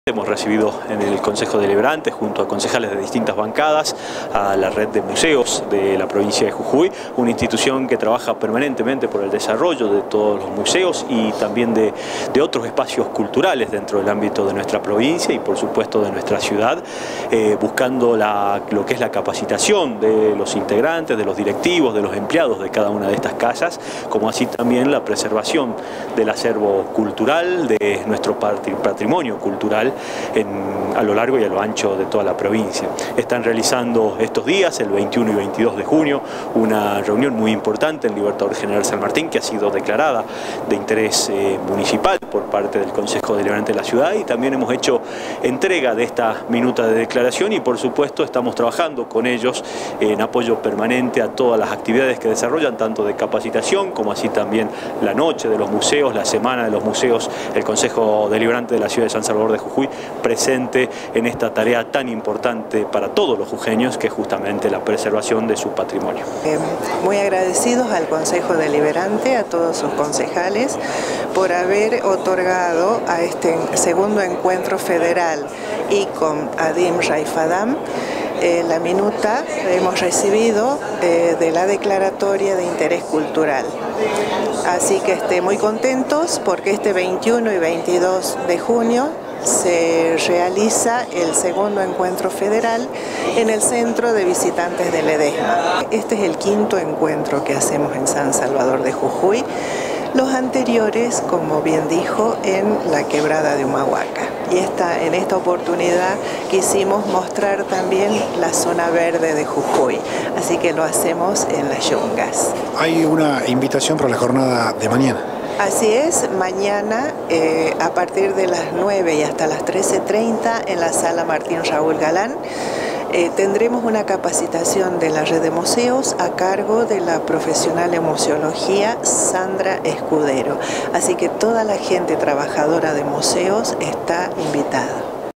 Reunión con la "Red de Museos" de Jujuy
18 de junio de 2019 - Concejo Deliberante de San Salvador de Jujuy